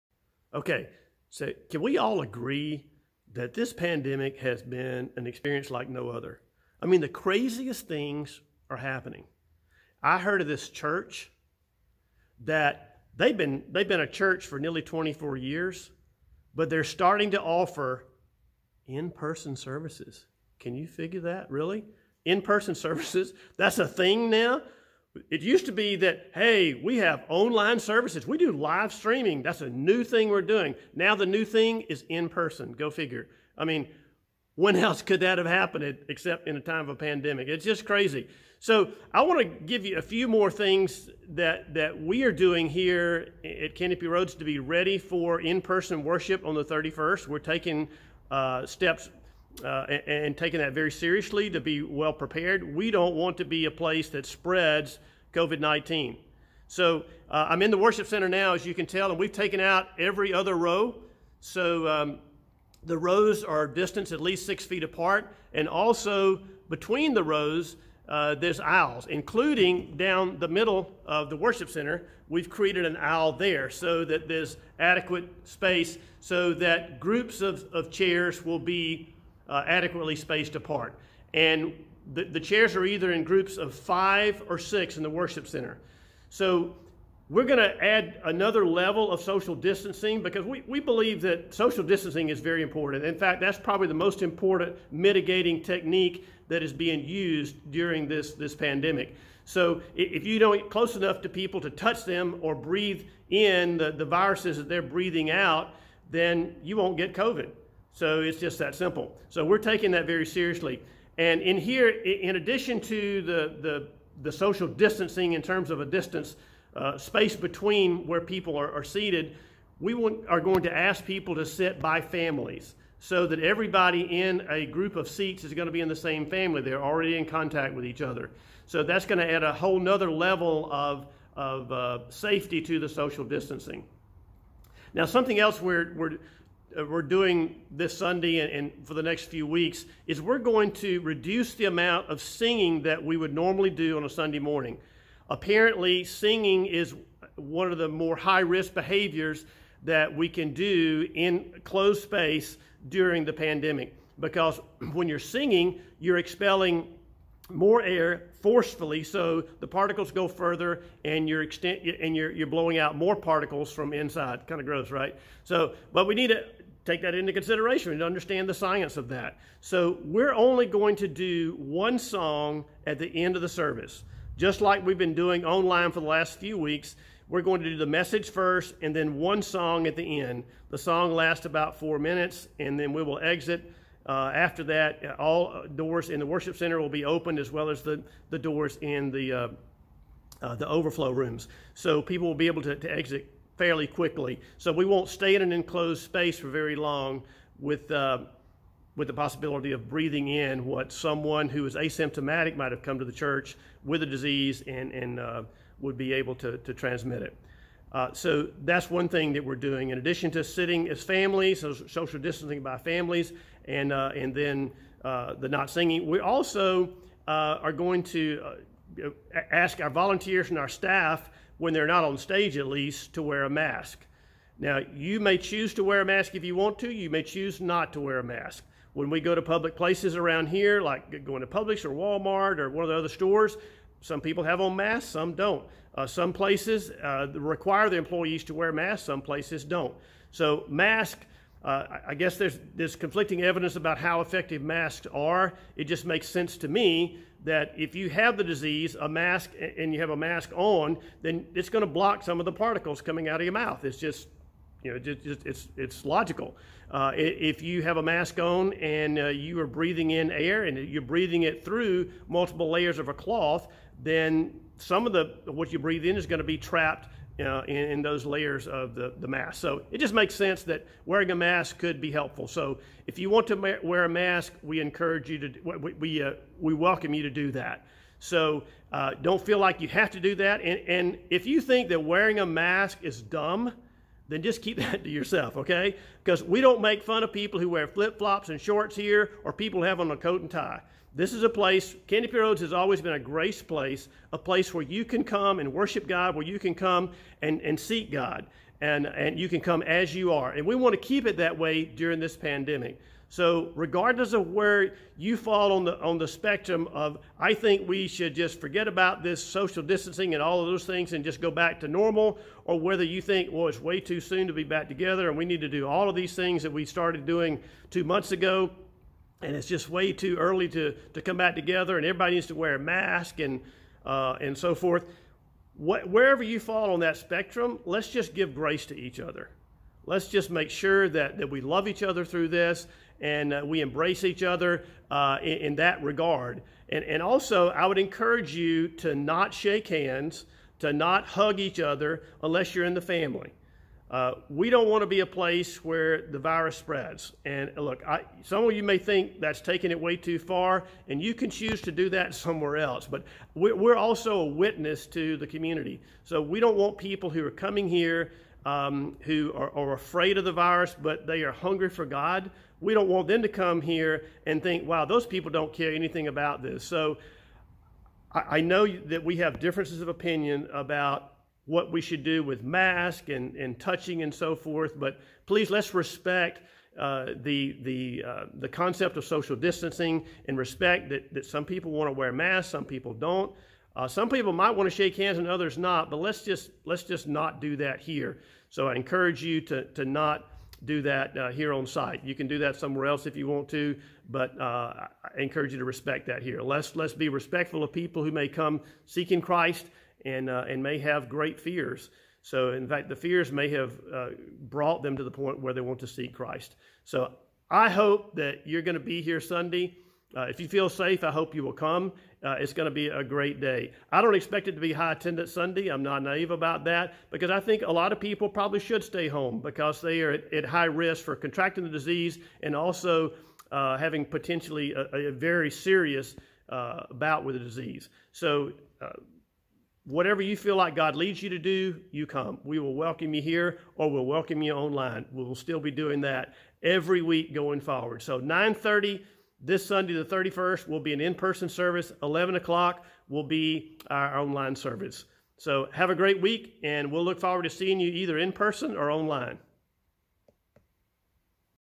How to Return Safely to In-Person Services
Pastor Matt details what CRBC is doing to create a safe environment for people to return to in-person services and what you can do to help be a part of the solution.